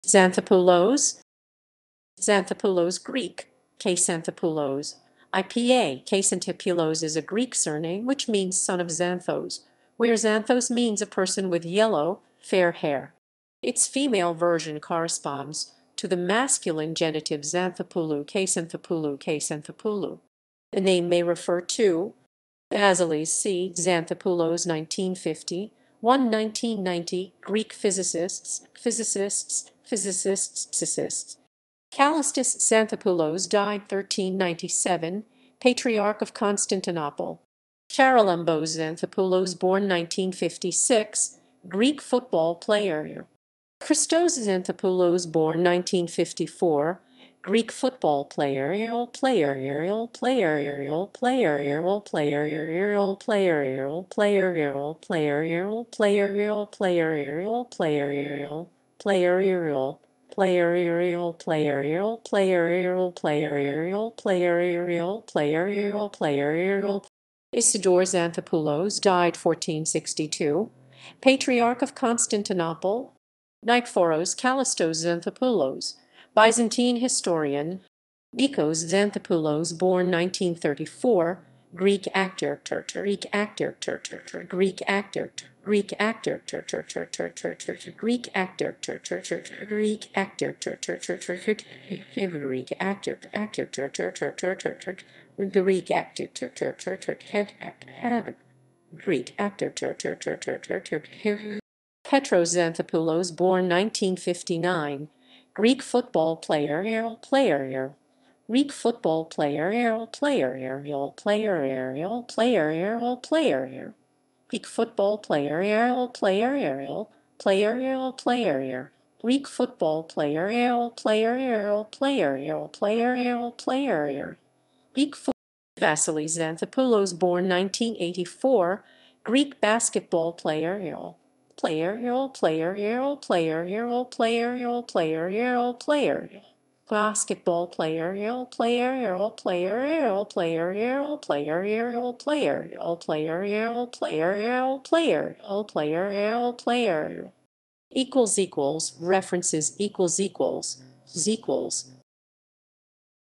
Xanthopoulos
Xanthopoulos (Greek: Ξανθόπουλος, IPA: [ksanˈθopulos]) is a Greek surname which means "son of Xanthos", where Xanthos means a person with "yellow, fair hair". Its female version corresponds to the masculine genitive Xanthopoulou (Ξανθοπούλου [ksanθoˈpulu]). The name may refer to:

Basilis C. Xanthopoulos (1951–1990), Greek physicist

== References ==